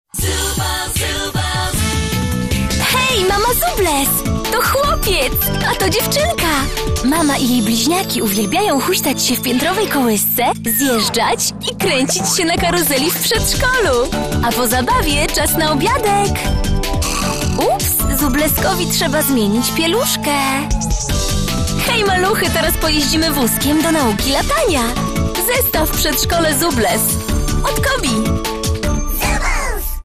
Zoobles - Przedszkole (30s)
W Zooblesowym przedszkolu jest mnóstwo atrakcji. Jest w nim kilka aktywnych miejcs do zabawy.